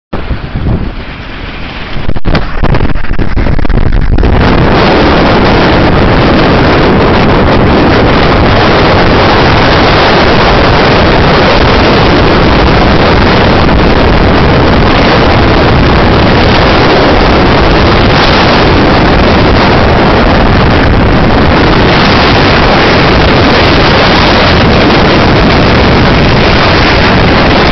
Crazy winds on the Howe sound
On my way to th Sunshine coast, wild winds, maybe 80 km/h